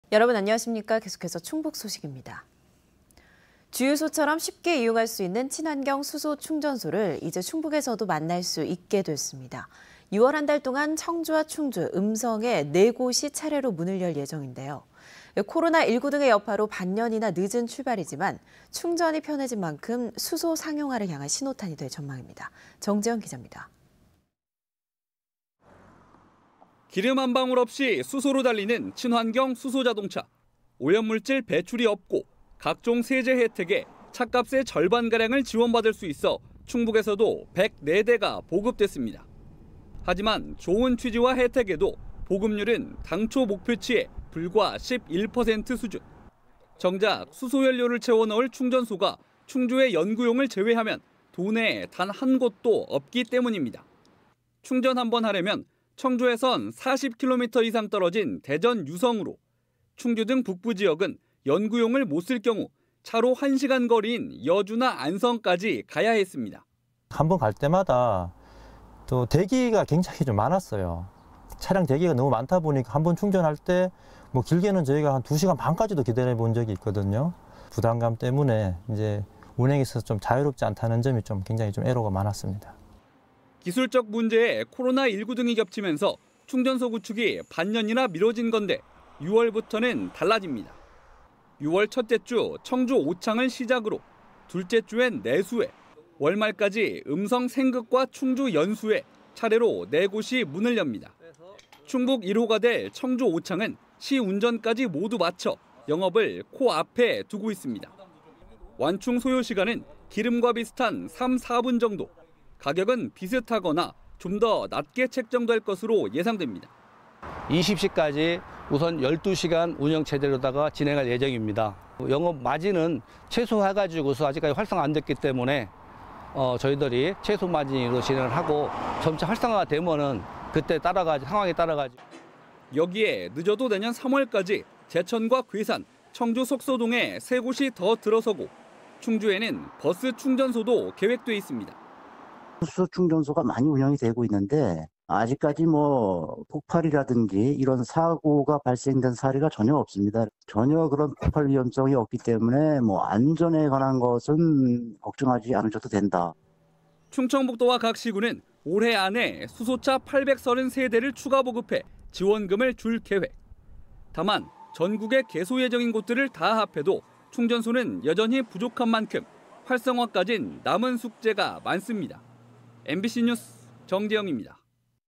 '6월에만 4곳' 수소 충전소 가뭄 해갈ㅣMBC충북NEWS
주유소처럼 쉽게 이용할 수 있는
친환경 수소 충전소를 이제 충북에서도
만날 수 있게 됐습니다.

 6월 한 달동안 청주와 충주, 음성에
4곳이 차례로 문을 열 예정인데요.

 코로나19 등의 여파로 반 년이나 늦은
출발이지만 충전이 편해진만큼 수소 상용화를
향한 신호탄이 될 전망입니다.
정재영 기자입니다.
           ◀ＥＮＤ▶

           ◀ＶＣＲ▶
 기름 한 방울 없이 수소로 달리는
친환경 수소자동차.

(투명CG) 오염물질 배출이 없고,
각종 세제 혜택에
차값의 절반 가량을 지원받을 수 있어
충북에서도 104대가 보급됐습니다.

 하지만 좋은 취지와 혜택에도
보급율은 당초 목표치의 불과 11% 수준.

 정작 수소 연료를 채워 넣을 충전소가
충주의 연구용을 제외하면 도내에 단 한 곳도
없기 때문입니다.

 (완Cg)충전 한 번 하려면
청주에선 40km이상 떨어진 대전 유성으로,
충주 등 북부지역은 연구용을 못 쓸 경우
차로 한 시간 거리인 여주나 안성까지
가야했습니다.

 ◀ＩＮＴ▶노경훈/수소자동차 운전자(청주)
"(타 지역 충전소에) 한 번 갈 때마다 대기가
굉장히 많았어요. 차량 대기가 많다 보니까
한 번 충전할 때 길게는 저희가 2시간 반까지
기다려 본 적이 있거든요. 부담감 때문에
운행에 있어 자유롭지 않다는 점이 굉장히
애로가 많았습니다."

 기술적 문제에
코로나19 등이 겹치면서 충전소 구축이
반 년이나 미뤄진 건데 6월부터는 달라집니다.

(투명CG) 6월 첫째주 청주 오창을 시작으로
둘째주엔 내수에, 월말까지 음성 생극과
충주 연수에 차례로 4곳이 문을 엽니다.

 충북 1호가 될 청주 오창은 시운전까지
모두 마쳐 영업을 코앞에 두고 있습니다.

 완충 소요시간은
기름과 비슷한 3, 4분 정도, 가격은 비슷하거나
좀 더 낮게 책정될 것으로 예상됩니다.

 ◀ＩＮＴ▶김현일/충북 1호 수소 충전소 대표
"20시까지 우선 12시간 운영 체제로 진행할
예정입니다. 영업 마진은 최소화해서 아직까지
활성화 안됐기 때문에 최소 마진으로 진행하고
점차 활성화가 되면 상황에 따라서."

 여기에 늦어도 내년 3월 전까지
제천과 괴산, 청주 석소동에 3곳이
더 들어서고 충주에는 버스 충전소도 계획돼
있습니다.

 ◀ＳＹＮ▶이일우/충북도청 기후대기과장
"수소 충전소가 많이 운영되고 있는데 아직까지
폭발이라든지 사고가 발생한 사례가 전혀
없습니다. 전혀 폭발 위험성이 없기 때문에
안전에 관한 것은 걱정하지 않으셔도 된다."

 충청북도와 각 시·군은
올해 안에 수소차 833대를 추가 보급해
지원금을 줄 계획.

 다만 전국의 개소 예정인 곳들을 다 합해도
충전소는 여전히 부족한만큼 활성화까진 남은 숙제가 많습니다. MBC뉴스 정재영입니다.